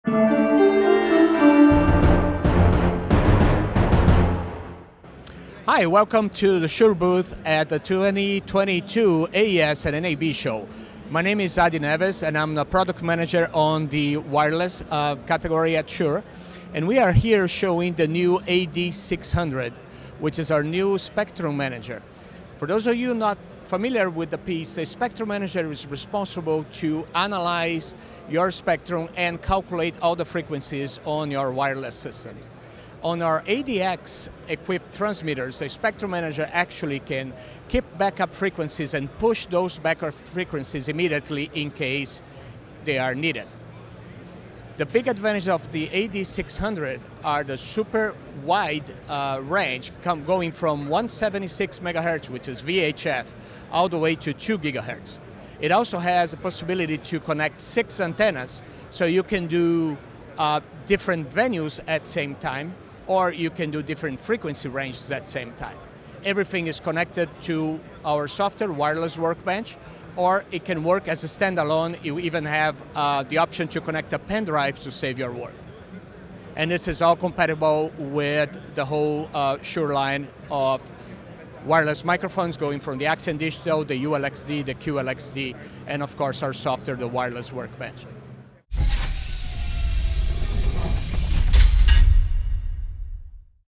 Shure Axient Digital AD600 Digital Spectrum Manager
Shure Axient® Digital AD600 Digital Spectrum Manager delivers real-time, wide-band spectrum scanning and monitoring from 174 MHz to 2.0 GHz, spectrum analysis, and frequency coordination in a single rack unit. Six antenna connections deliver multiple coverage options while Dante connectivity provides advanced audio monitoring of your network. Use the USB port to export, import, or save backup scans, event logs, and other important data. The AD600 is an essential addition to your Axient Digital professional audio setup.

The Shure Axient® Digital AD600 Digital Spectrum Manager is a powerful, next-generation tool for planning and managing frequency coordination in professional audio applications. Use Guided Coordination menus to plan, RF scan, and deploy frequencies to your entire system, or dive deeper for complete control. Additional tools are available to analyze the spectrum, capture data, and perform site RF surveys. Monitor demodulated audio from your network using Dante Browse and listen over Dante features. The AD600 features a large, full-color 6.6-inch front panel screen making viewing information easy while real-time scanning and monitoring of RF activities keeps live information available when you need it most.